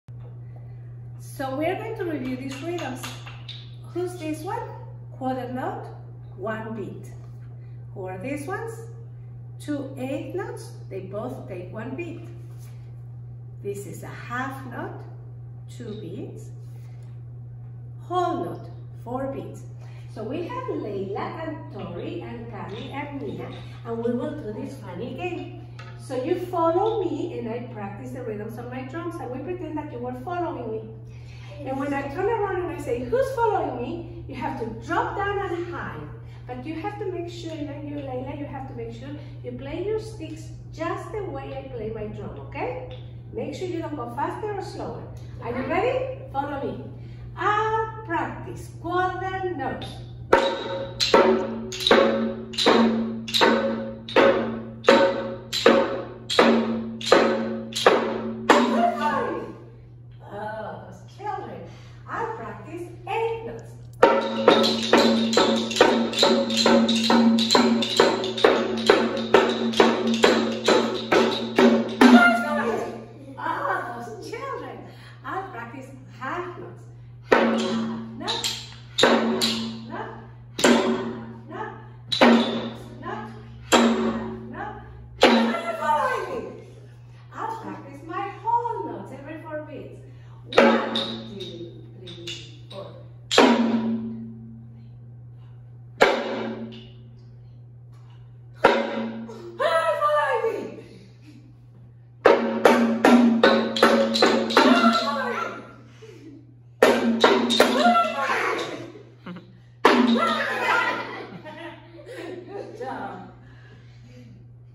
THEORY 1 1st Semester. Locomotion 'Who is following me" 4 rhythms
THEORY 1 1st Semester.
Locomotion 'Who is following me" 4 rhythms: Quarter note; Eighth notes; Half note; Whole note.